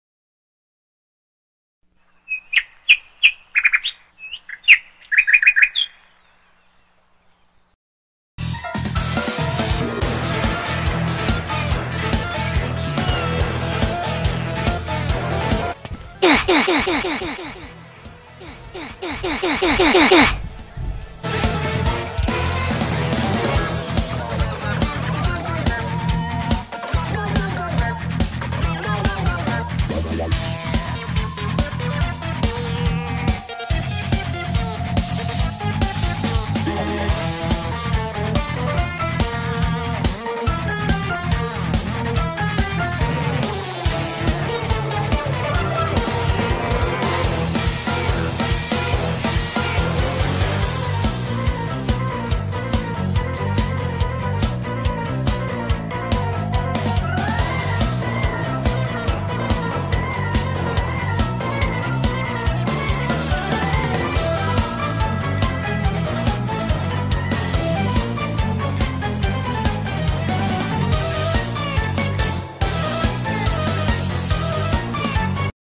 ROBLOX REMOVED THE OOF SOUND…
So an update was released on Roblox yesterday, which removed the iconic "OOF" sound and replaced it with a death sound that according to many Roblox players, sounds worse. So why was the iconic sound that all of us know about removed, and will Roblox ever bring it back?